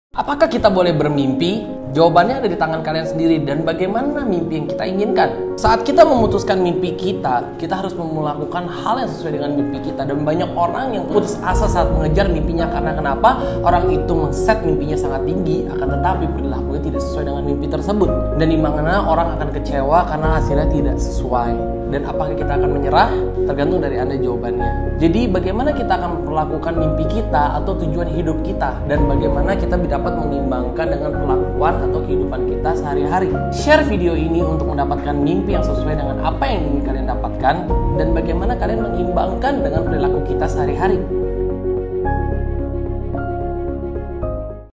MIMPI KETINGGIAN tapi NO ACTION???
Punya mimpi setinggi langit tapi tidak berusaha untuk menggapainya?
Mimpinya yang ketinggian atau kamu yang tidak mengambil ACTION?

Mari simak tips dari Mr. Antonius Arbian , untuk belajar apa saja yang menjadi hambatan kita dalam menggapai MIMPI kita!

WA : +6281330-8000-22